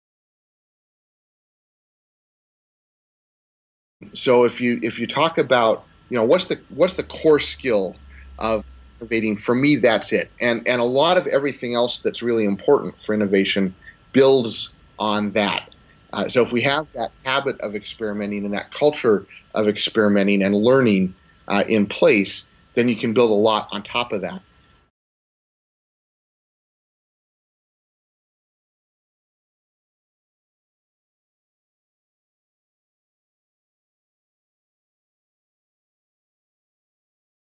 Defining Innovation Podcast Pearl | #003 Tim Kastelle
Tim Kastelle is the Senior Lecturer in Innovation Management at University of Queensland. His Discipline of Innovation blog is a wonderfully rich source of ideas, articles and discussions (follow him on Twitter via @timkastelle).

This is simply a little visual pearl from the whole interview which you can listen